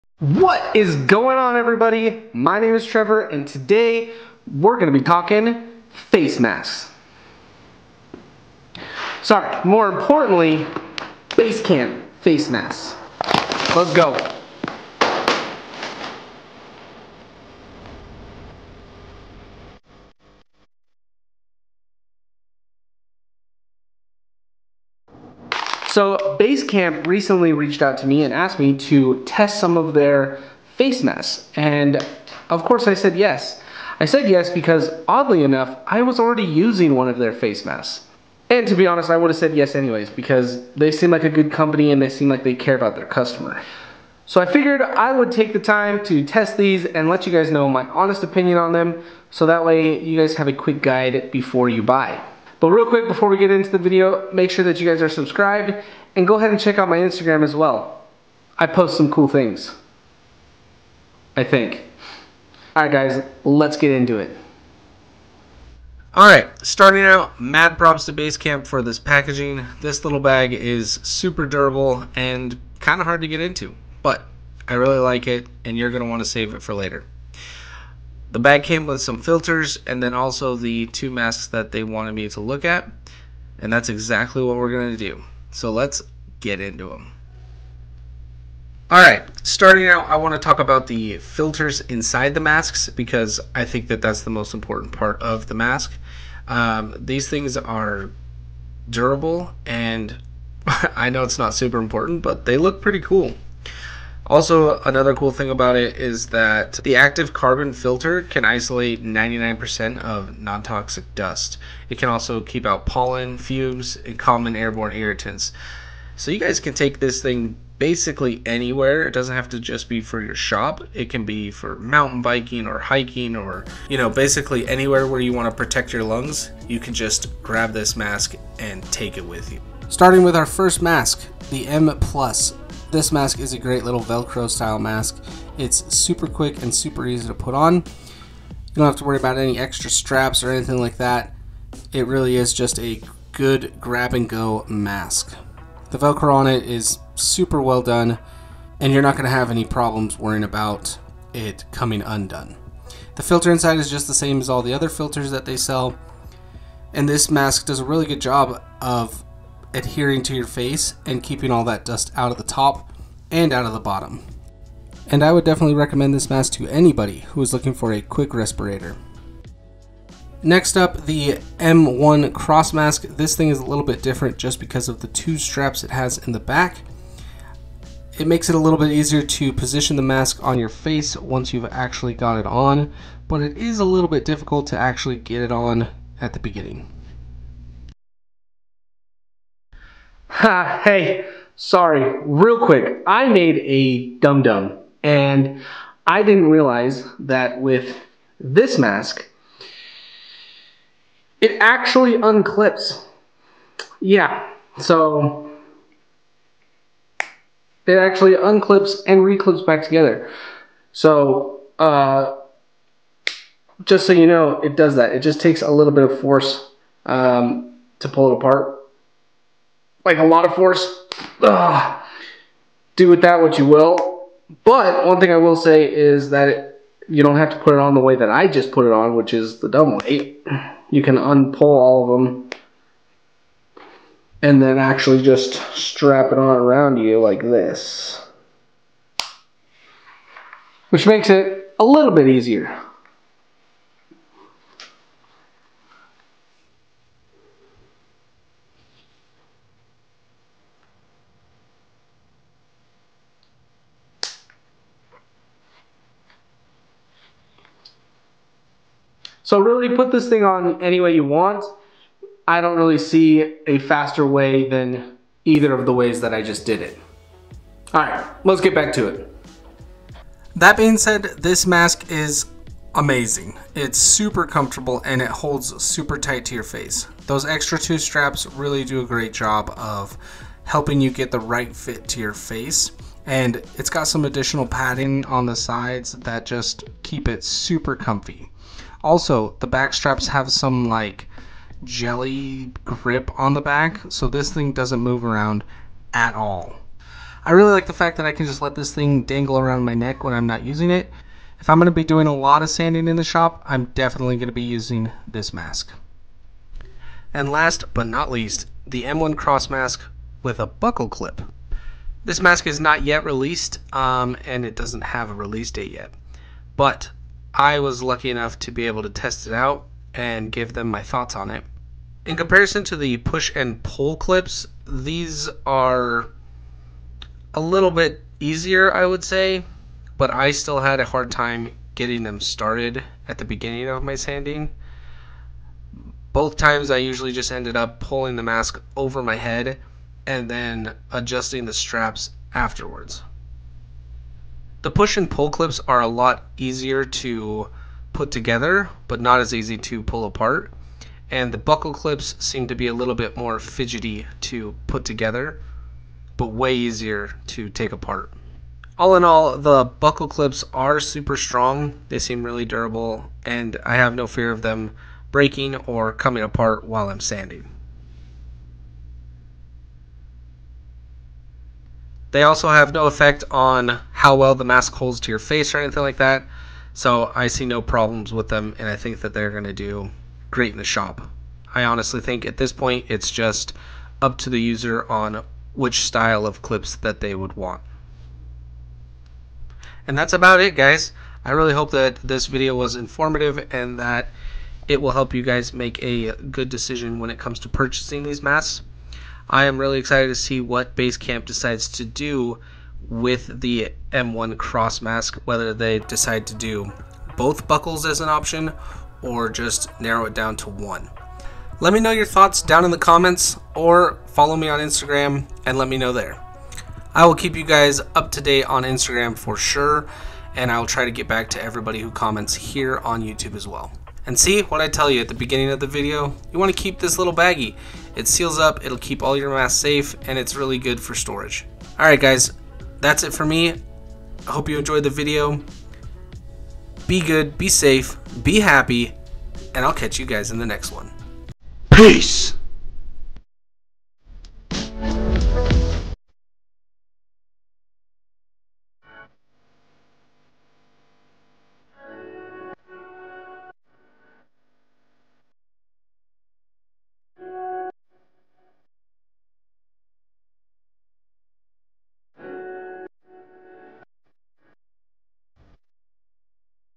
Basecamp Mask review
Healthy lungs are very important. Taking care of them doesn't have to be hard. A simple mask will protect your lungs and keep you strong and healthy!

In this video we are going to be going over the Base camp M plus mask and the M cross mask. ( not yet released.)
Basecamp sent  me the two M cross masks go try out and give them my thought. I am also lucky enough to be able to share them with you!

Let us know what you think and which mask you will be ordering for your shop!

Look what I made! :